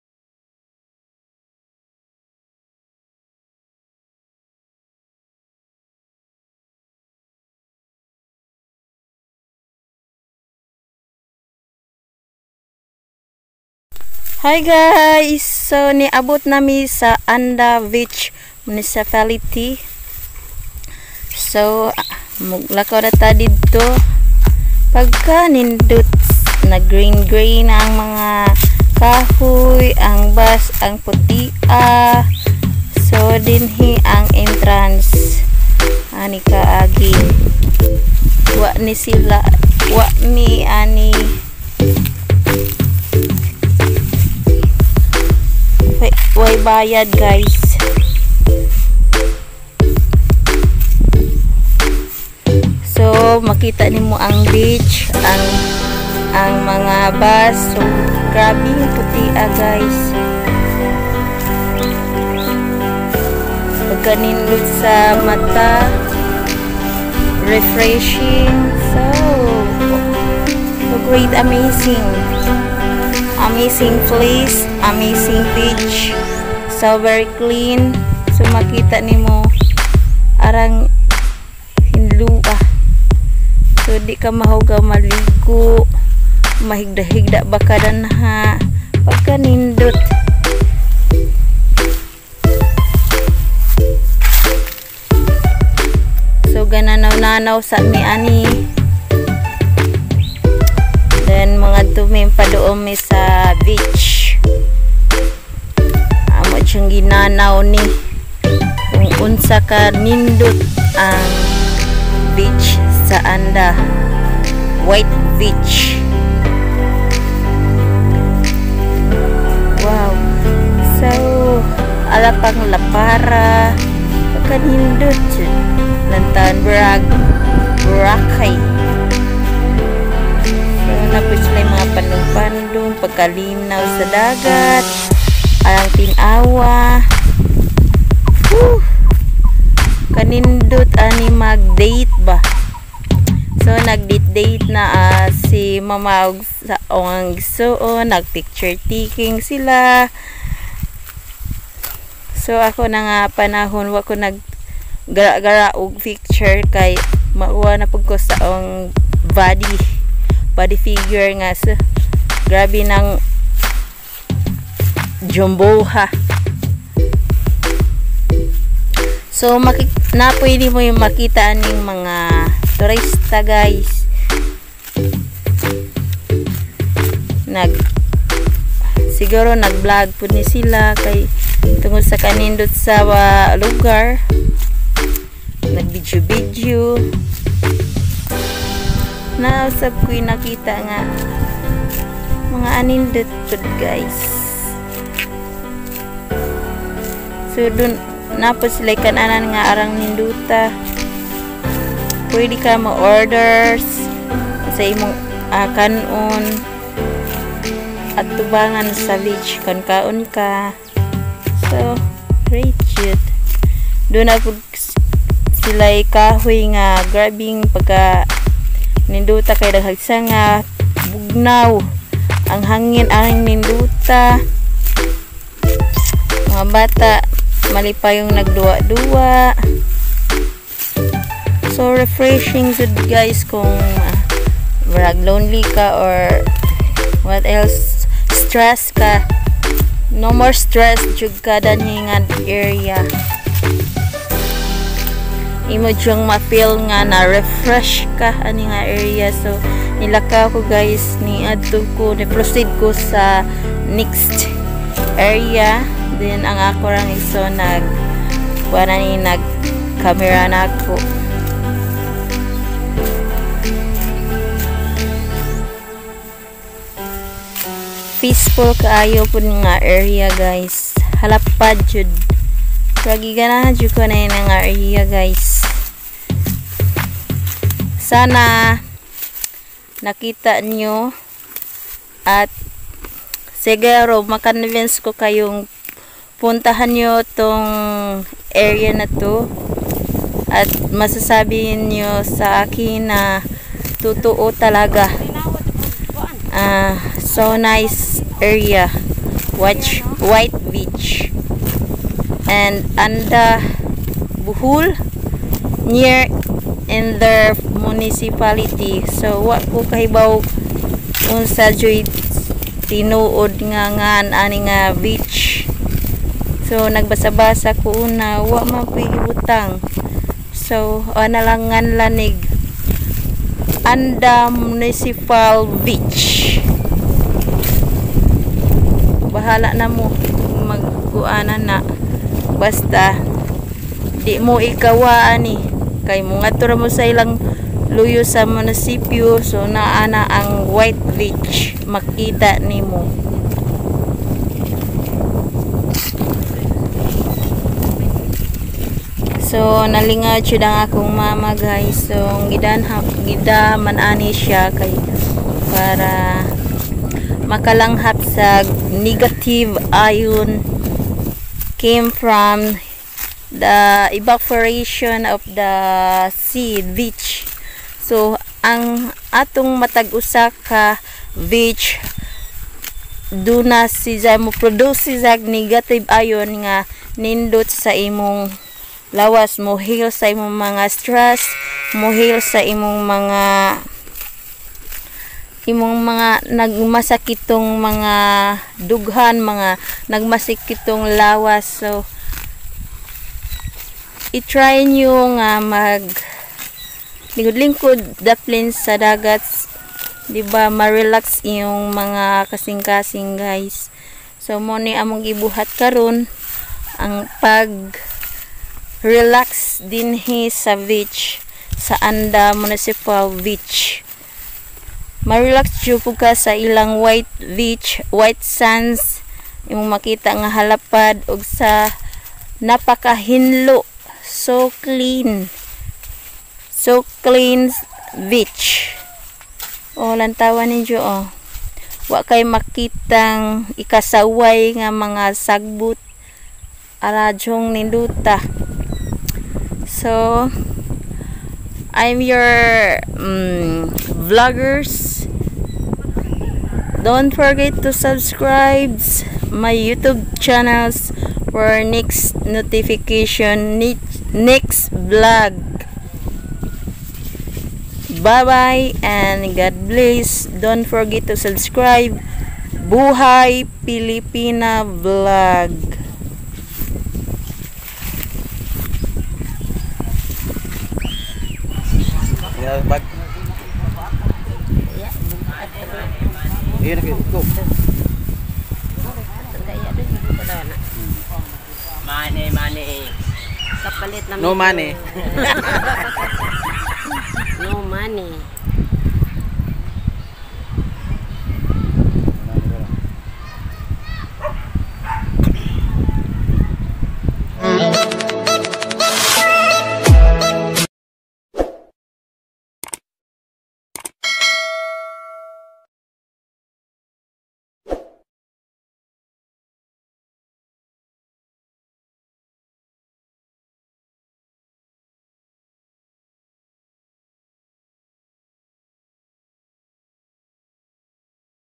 VISITING AND BEACH BOHOL PHILIPPINES
Hi I'm filipina vlogers. I visit ANDA BEACH together with my mom and my brother .  you know ANDA is a fifth class municipality located 99 kilometers away from Tagbilaran city, Bohol's capital  people say that it would take 2 hours by private transportation from tagbilaran to ANDA. so I guess it would take more time via a public transport like bus. don't fret about the long drive thought, since from Tagbilaran to ANDA, one would take the exterior national highway. thus, along the way one could  enjoy the good sight of Bohol's ocean the right, and mountain ranges tress and old churches on the lift. by the way my reason why I visit Beach now is for mom breathe the negative ions of the sea askyou don't know  the oceans give usa negative ions that makes our body fresh and healthy, it refreshing our mindand heart.thanks for watching mga friends don't forget to subscribe me Buhay Filipina vlog